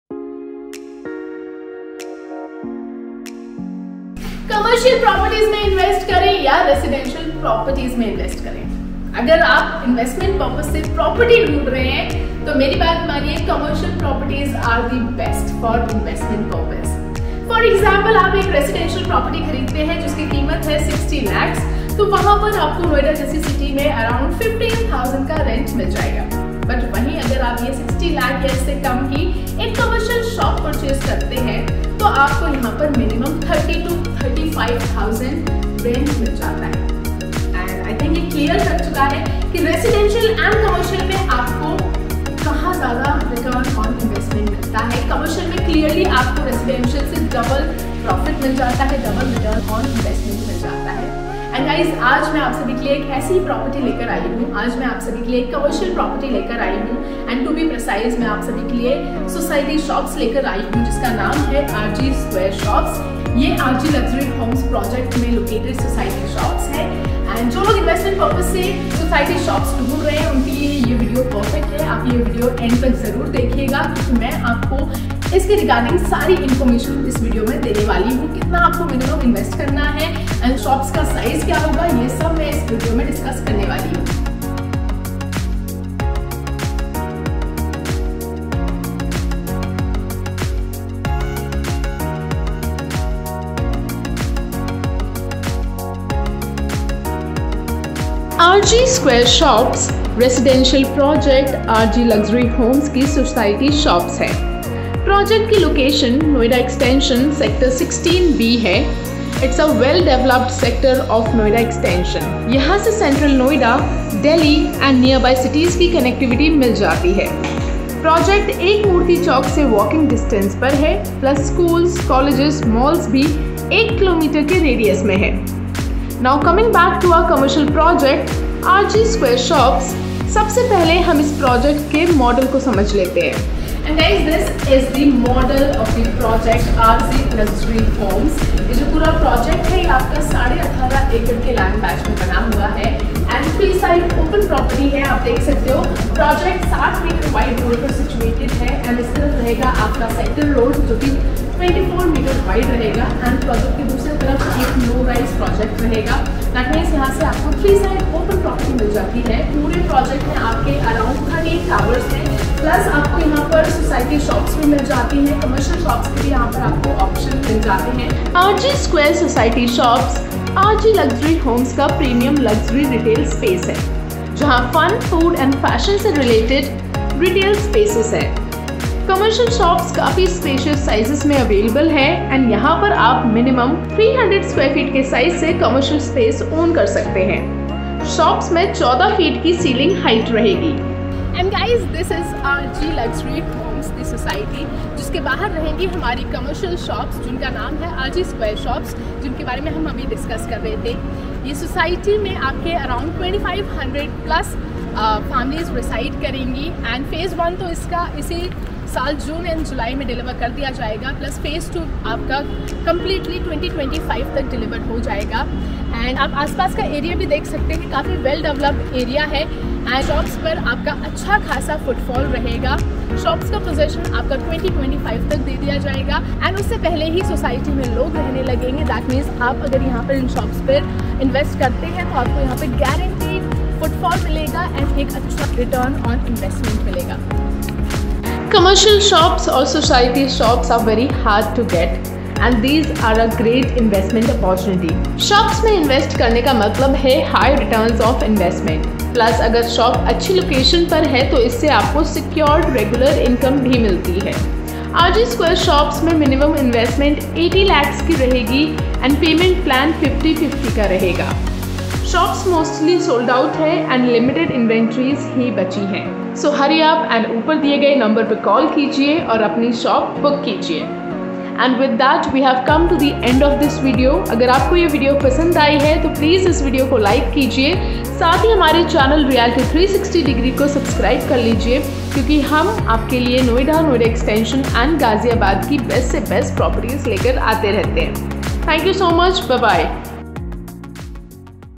RG Square Shops | Premium Retail Space | Noida Extension | RG LUXURY HOMES
RG Square -An epitome of Premium Retail.

*RG Square is One of Greater Noida West’s Best Retail Market featuring around 90 Ground Floor Shops.
*A one-stop destination for Fun, Food, and Fashion.
*Luxury and spacious Commercial Spaces Located on a 24 MT Wide Road near Gr Noida West One Murti Roundabout.
*Sizes of Retail Shops start from 300 sq. ft.
*14 Feet Roof Height.

RG Square is the part of 18.5 Acres RG Luxury Group Housing Society having aprrox 2800 Units & 13 Towers.

RG Square will serve the entirety of Greater Noida West with its luxury and affordable shopping options.